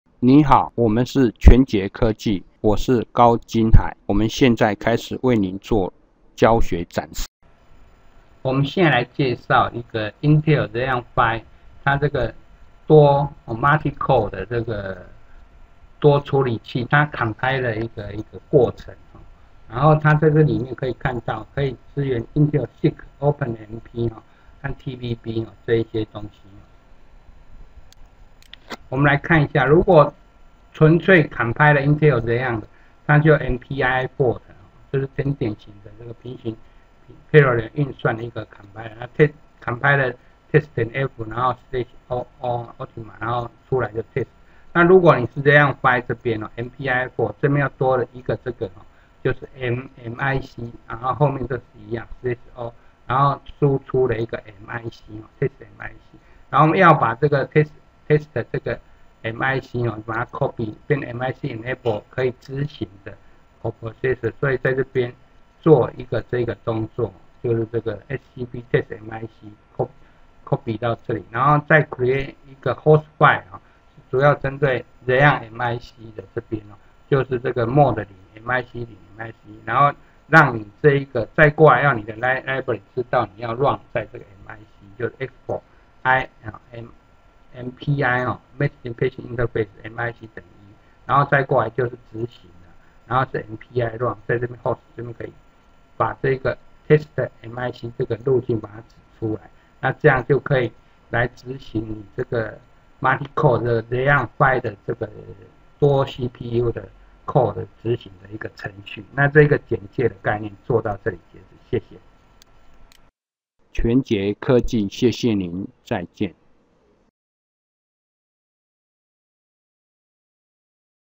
Intel Xeon Phi compiler
Tutorial for :

How to run
• Compile your code for the Intel® Xeon node
$ mpiifort test.f --o test
• And for Intel® Xeon Phi™ Coprocessor
$ mpiifort --mmic test.f --o test.mic
• Copy the MIC-enabled executable to the coprocessor
$ scp test.mic mic0:/home/user/test
• Create your hosts file (Xeon+MIC)
$ cat hosts
node0
mic0
mic1
• Let the library know you're planning on running on MIC
$ export I_MPI_MIC=1
• Run your application (from the Xeon)
$ mpirun --f hosts --n 4 /home/user/test.mic

Intel 授權經銷商
SoftHome  International ; Software for Science
13F, NO. 55, SEC.1, CHIEN KUO N-ROAD, TAIPEI, 10491,TAIWAN
臺北市中山區建國北路一段五十五號十三樓